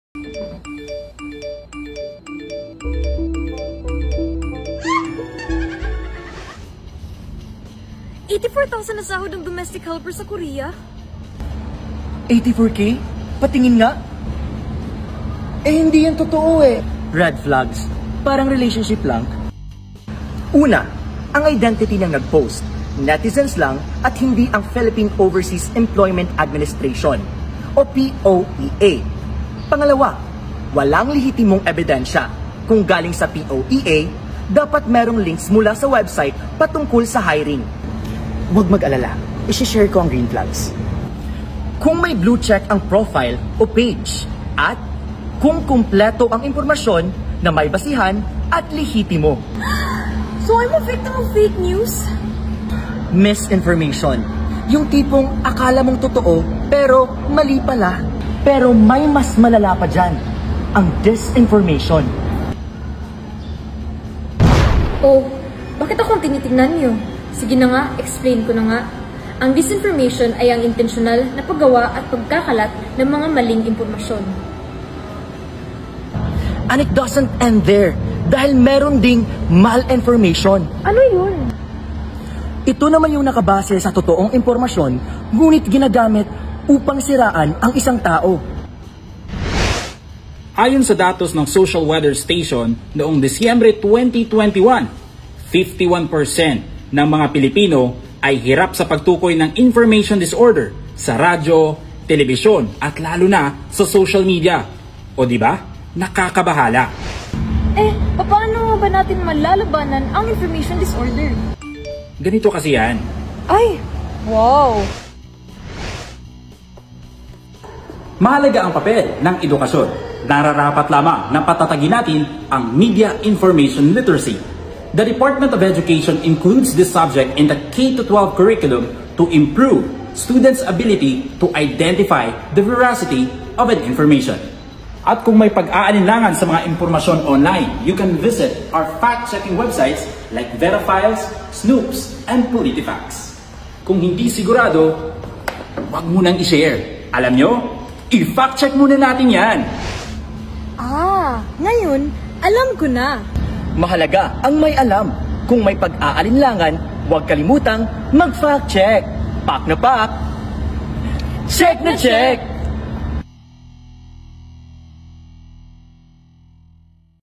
PAK NA PAK, CHECK NA CHECK! - 1st Place PAK! CHECK VIDEO MAKING CONTEST | Media Civics Lab
A group of undergrads from Cebu Normal University collaborated to produce an insightful Fact-Check video. Their video focuses on teaching viewers how to spot "Red Flags" in online job postings, while also shedding light on the key indicators or "Green Flags" that can help distinguish legitimate job opportunities from potentially deceptive ones.

The entry won 1st Place during The Pak! Check Video Making Contest which is part of the activities of the Media Civics Lab Fact-Checking Academy. This Academy equip influencers, vloggers, bloggers, and content creators with the skills necessary to detect and combat fake news effectively.